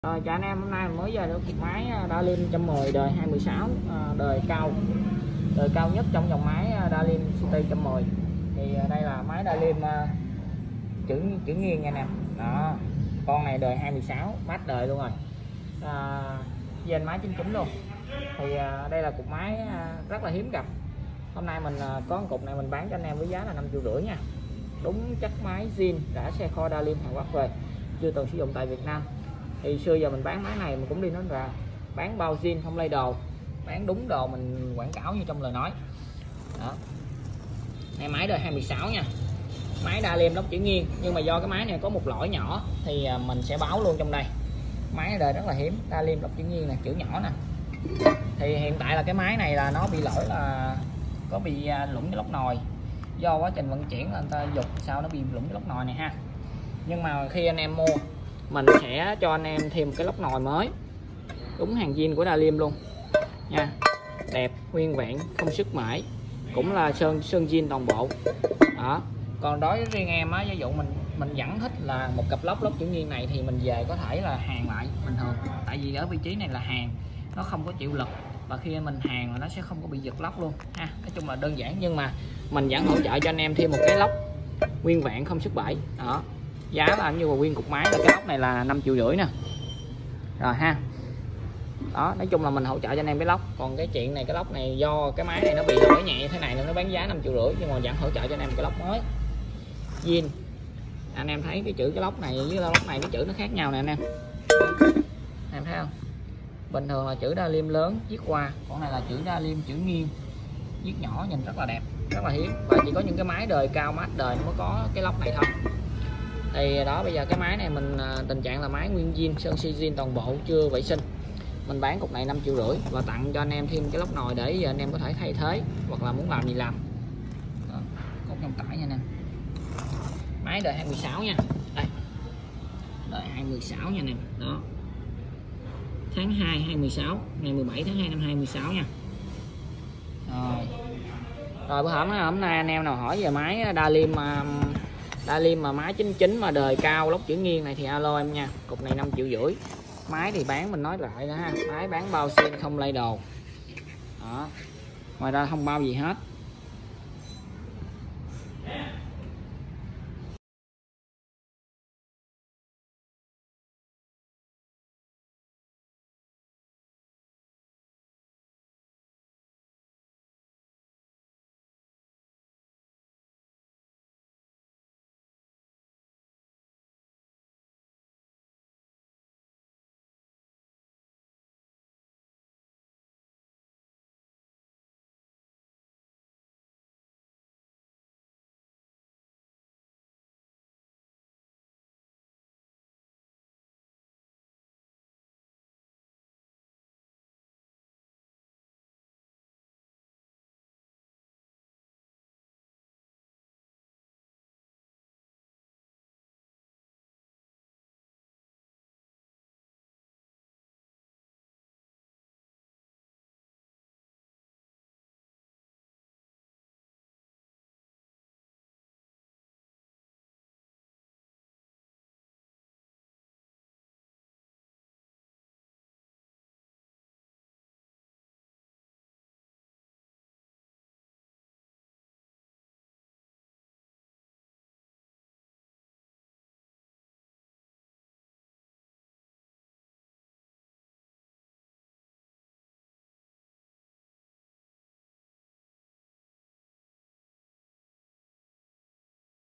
⏰ ĐÃ BÁN 💥 Máy Citi Plus 110 Đời 2016 Ống Hơi Dên Má 99 👉Giá : 5,5 Triệu / Máy
⏰ 23/10/2021 HÀNG HIẾM

💥  Máy Citi Plus 110 Đời 2016 Ống Hơi Dên Má 99 Cực Đẹp Zin rã xe kho Nội Địa Hàn Quốc

👉Giá : 5,5 Triệu / Máy

🆘 CÓ LỔI LỔ LÓC BÊN NỒI DO VẬN CHUYỂN NHƯNG SẼ BÙ CHO AE 1 LỐC NỒI ZIN DEALIM ĐỂ THAY THẾ 👌

⛔️Bán chỉ Bao máy Zin không lai đồ ngoài ra không bao gì hết

💯Đúng chất máy Kho Hàn Quốc về nên cực đẹp 👉Ae vui lòng đừng đem so sánh với những cục Máy Citi 110 mà đã bị mấy A Campuchia chạy nát bét rồi bán về VN 👌👌

+ Agribank, CN Bình Tân, HCM
+ Đông Á Bank, CN Trường Chinh , HCM
+ Sacombank , CN Hốc Môn, HCM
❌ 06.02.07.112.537